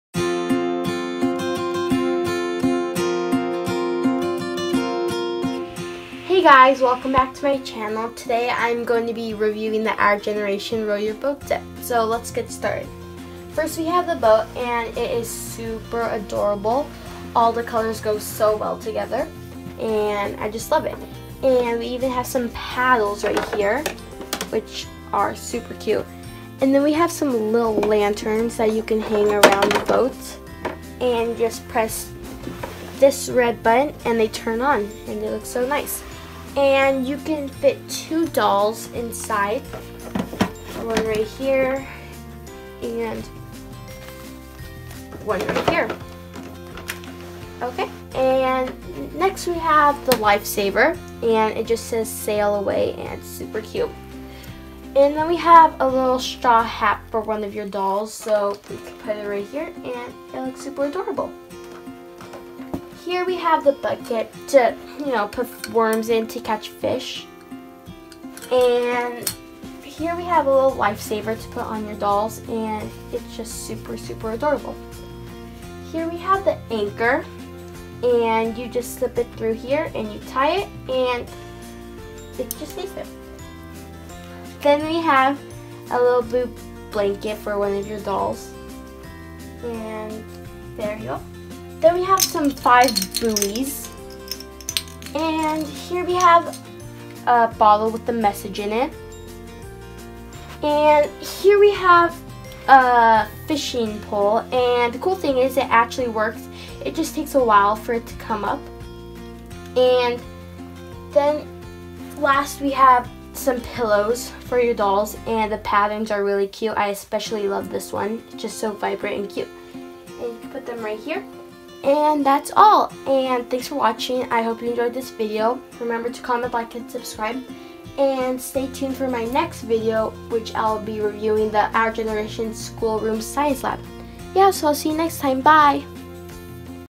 Reviewing the Our Generation Row Your Boat set
In my previous video, I said I would review this item and I did! It's really fun and maybe I'll film a stopmotion with it. Anyways I hope you enjoyed this video.